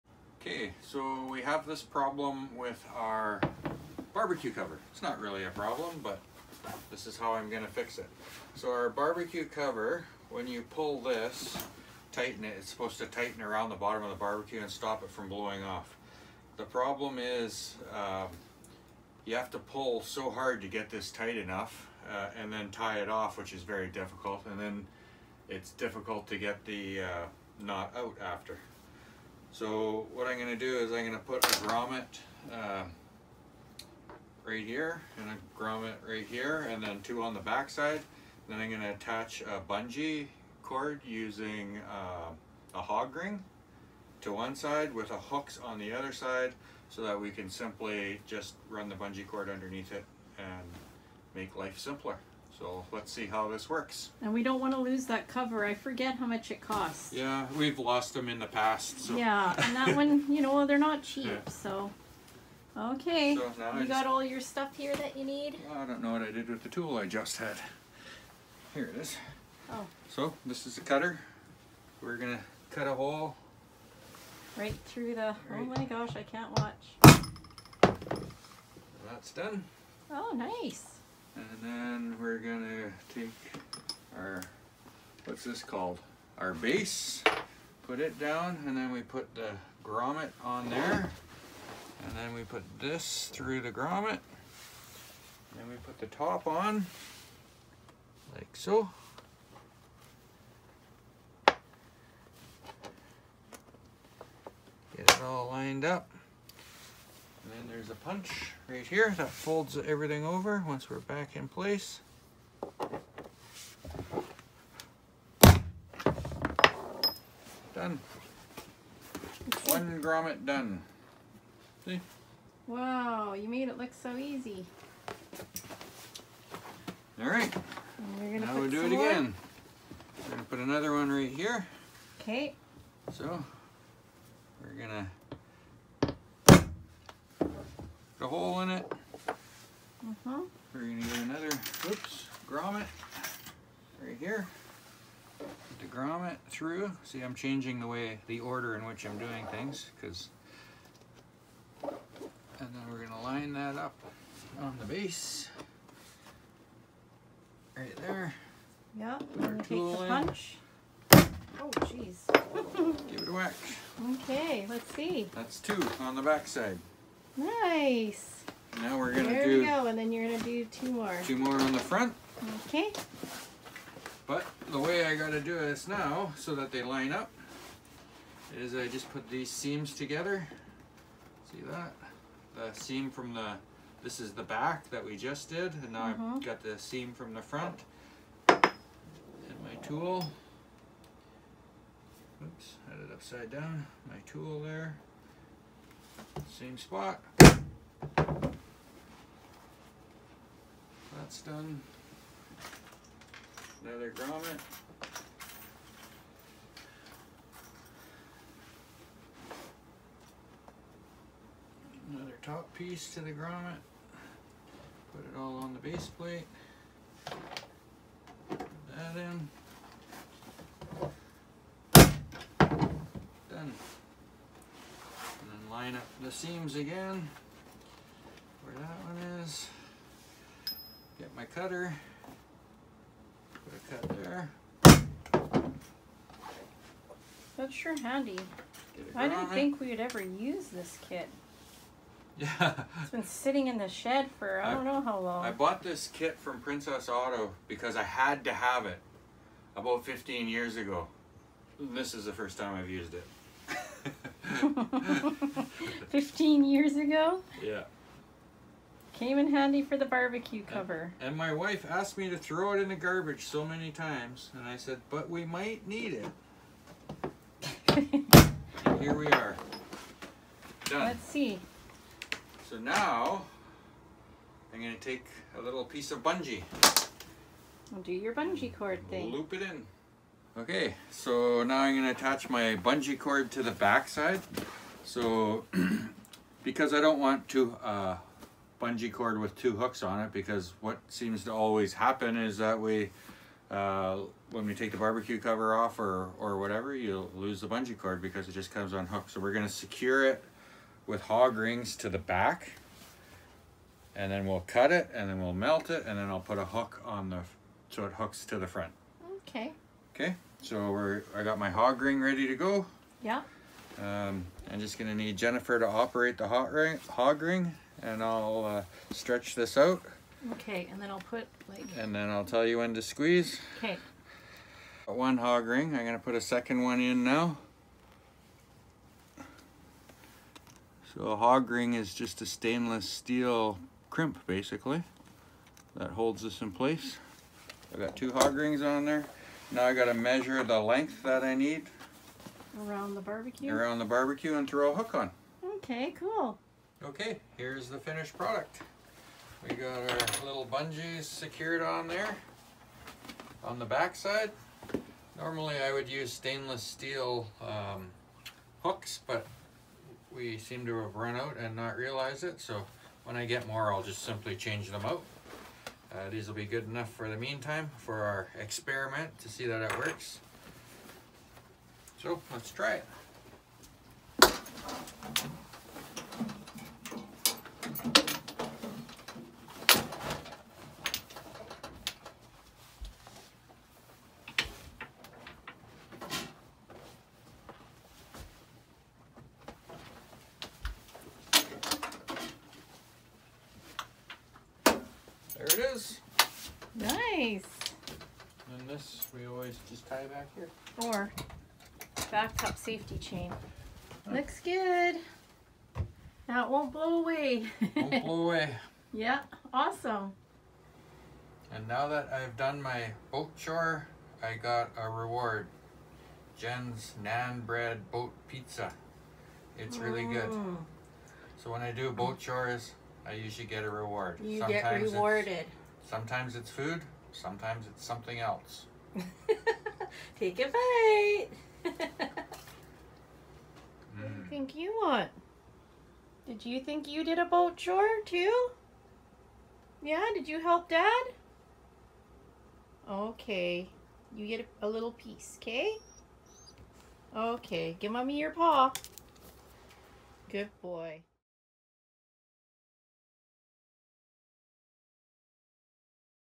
We Are Not Going To Lose This BBQ Cover to High Winds!
In the past we have lost a couple of BBQ covers due to high winds, rough seas and well we won’t talk about my knots (Jen) Lol and this time the bbq cover is getting a seatbelt!